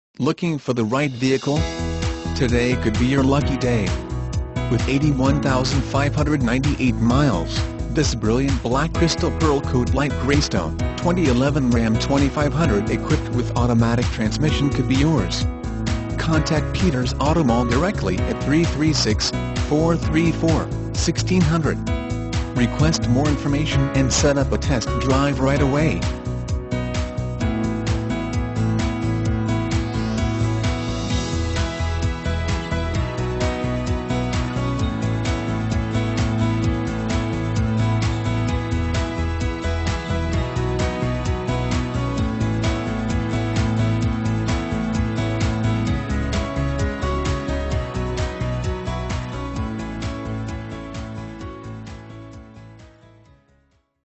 2011 Ram 2500 High Point North Carolina 20465
2011 Ram 2500  - $29927

Looking for the right vehicle? Today could be your lucky day. With 81598 miles, this Brilliant Black Crystal Pearl CoatLight Graystone, 2011 Ram 2500 equipped with Automatic transmission could be yours.